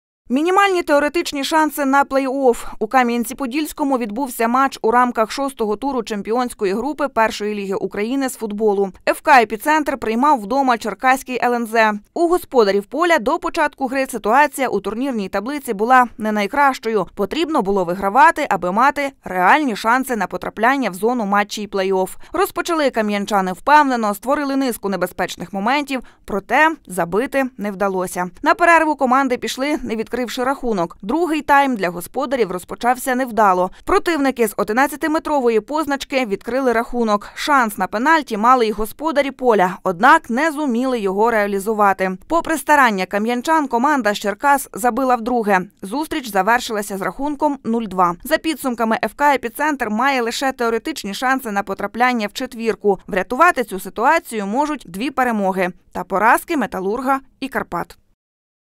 ФК "Епіцентр": шанси на плей-офф мінімальні
Шанси є, проте лише теоретичні. Кам'янець-Подільський "Епіцентр" зіграв свій матч 6 туру чемпіонату першої ліги України з футболу. Знову пограли і скоріше за все підвищення в класі не буде, принаймні цього року.